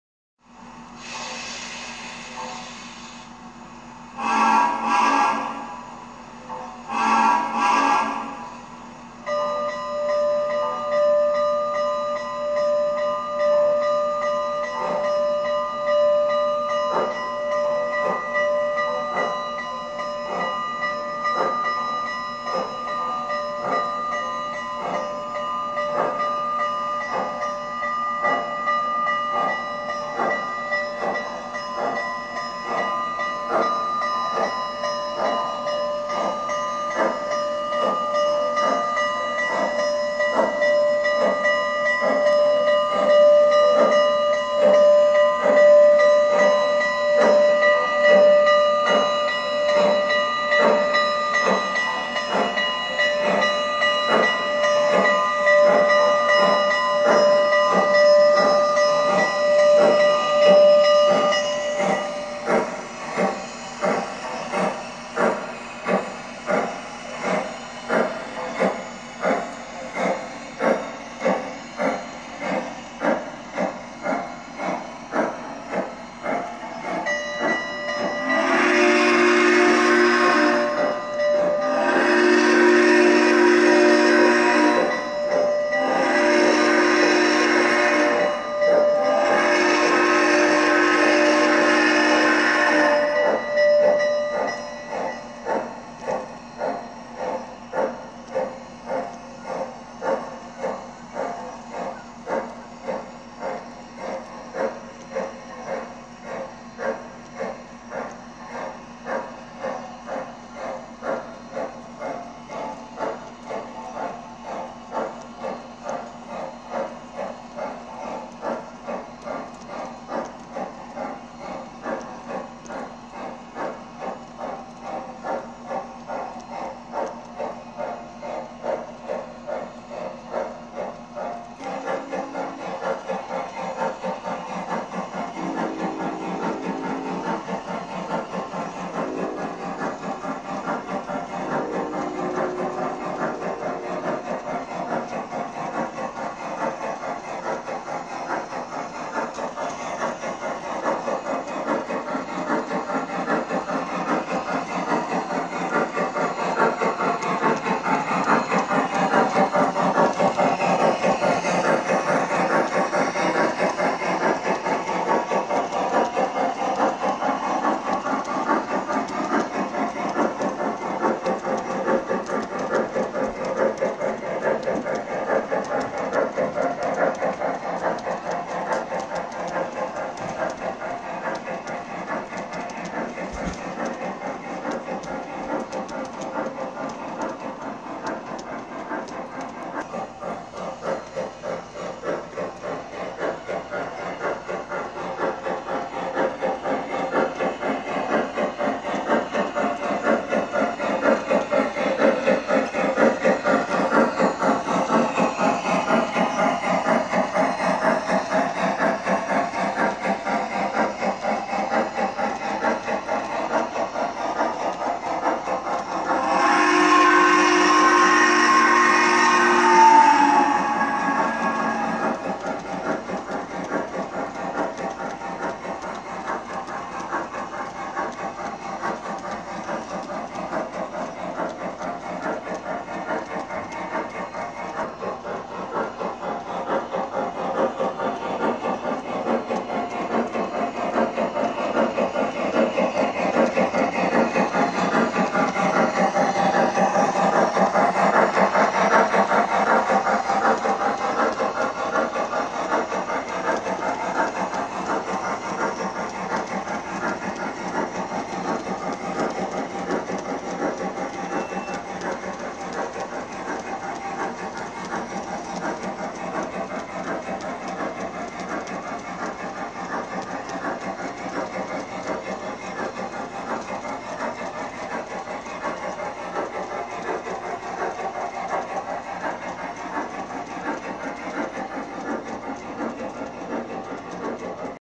MTH G Scale American Freedom Train/ Imagination Station Kids On Track
This video displays the sight, sound and smoke of MTH's complete G scale American Freedom Train on the Imagination Station Kids On Track Model Railroad. This one is a family favorite! We hope to make a ride on video soon, so be looking for it! We hope you like them both.